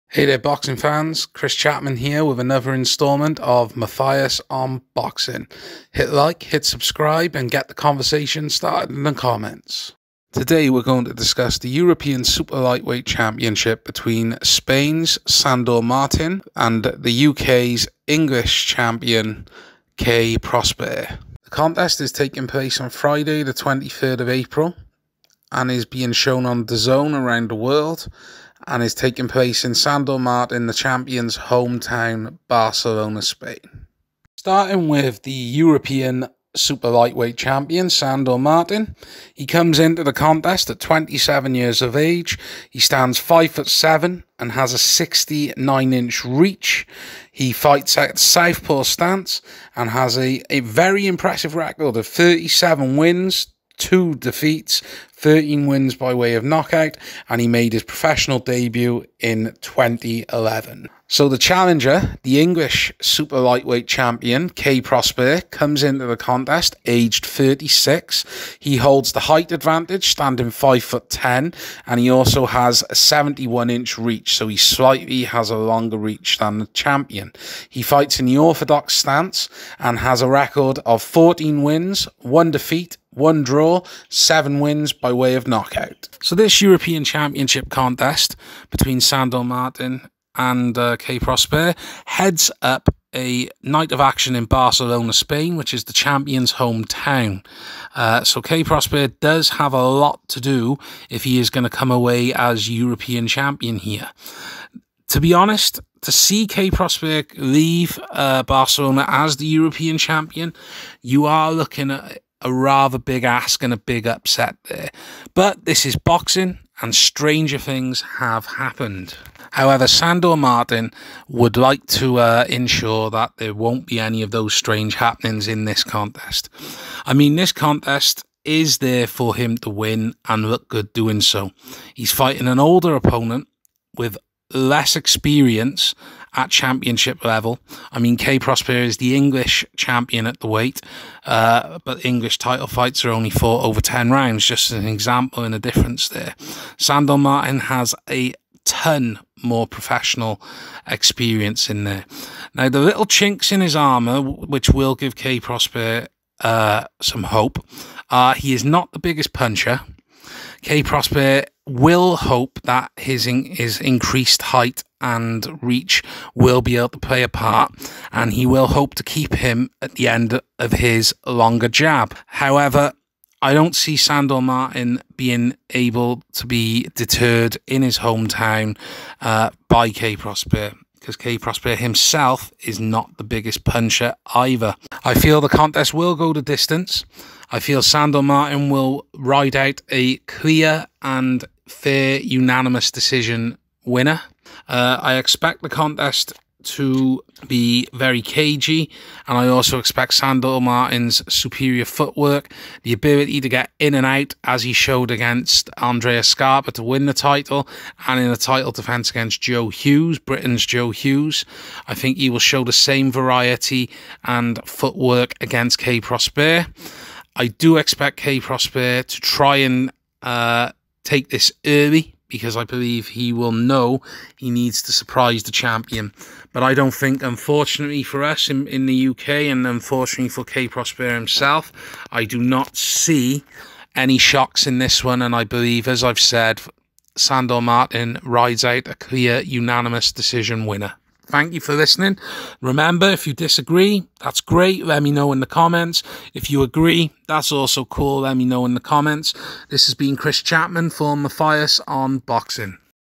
SANDOR MARTIN Vs KAY PROSPER FIGHT PREDICTION! Live on dazn April 23
Sandor Martin takes on Kay Prosper as he defends his European Super Lightweight crown live on April 23 on Dazn